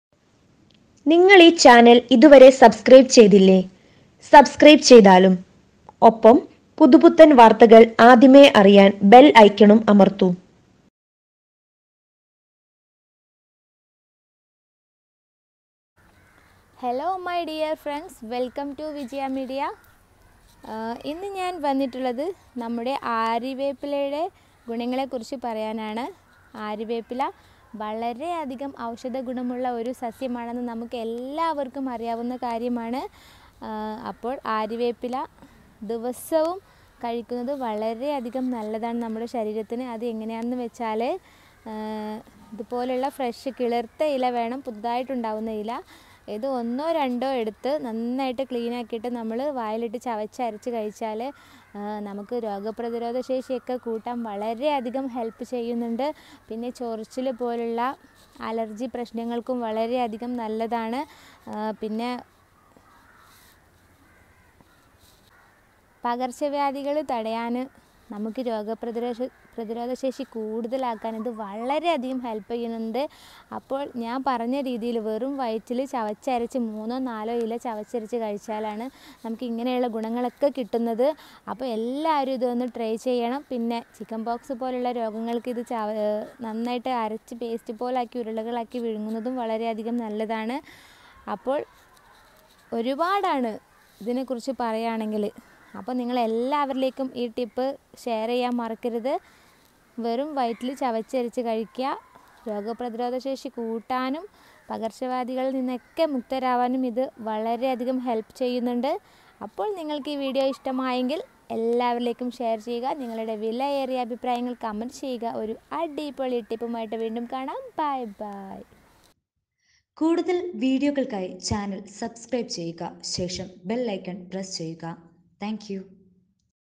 ഈ മരം വീട്ടില്‍ ഇല്ലെങ്കില്‍ നഷ്ടം ഇപ്പോള്‍ തന്നെ വീട്ടില്‍ വളര്‍ത്തൂ | Nee tree benefits | Aryavepp
Hi vijaya media friends, this video about neem tree. Aryavepp tree benefits.